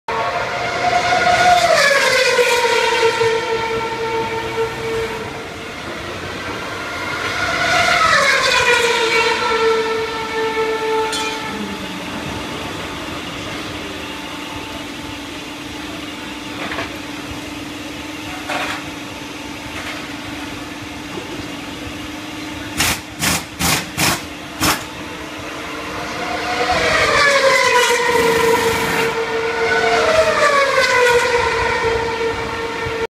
Inside F1 Team Marussia garage in Austin
With Armin Strom at F1 races in Austin Texas